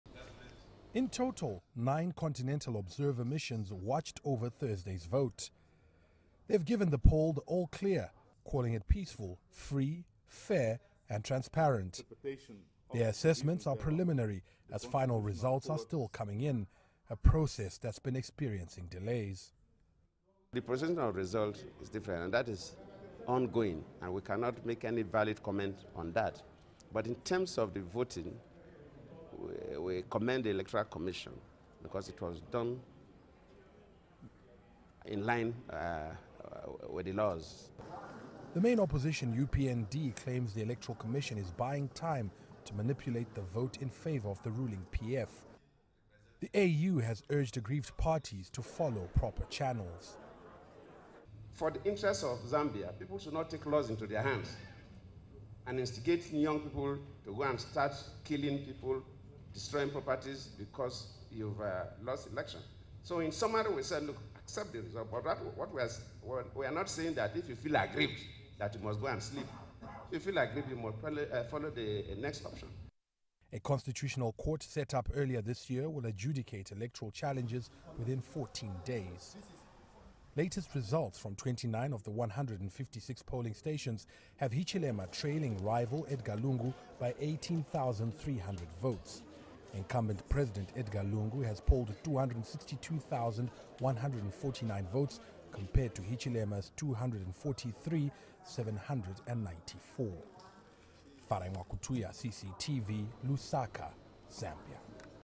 Zambia Elections: AU gives vote a clean bill of health
The African Union has said Thursday's election in Zambia satisfies the continental guidelines for elections. The AU election observer mission presented its preliminary election report Saturday. Former Nigerian president Goodluck Jonathan led the Mission. CCTV's Farai Mwakutuya has that story.